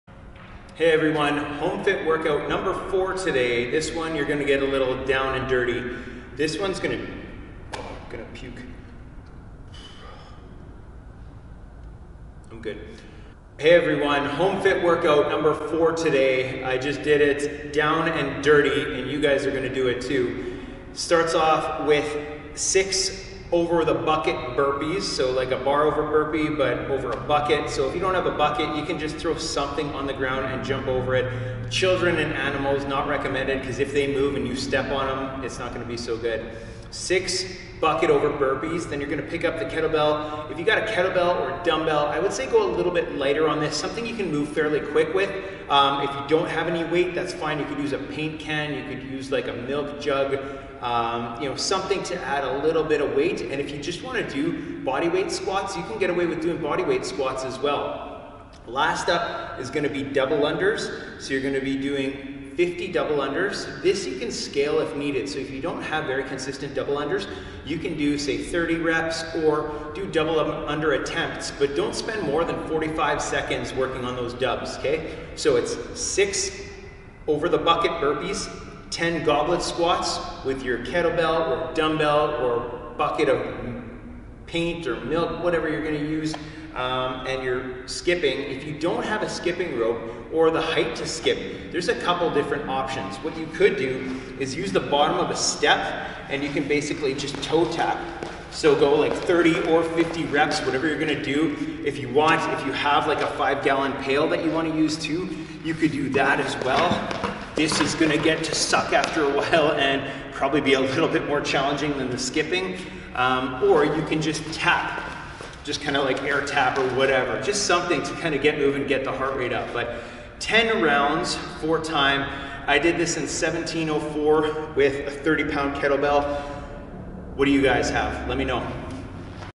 Starke HomeFit "Down N' Dirty"
Friday 03-20-20 HomeFit

10 RFT
"Down & Dirty"
6 Bucket Over Burpees
10 Goblet Squats 20/30#
50 Double Unders or Single Unders

*Use an object to jump over for the burpees
*Body Weight squats or some sort of weight can be used for the squats
*Toe taps on a step, box, table, chair or bench will work.

Start off slower because it starts to suck around the fifth round. Your last few rounds should be faster than the first few if you pace correctly.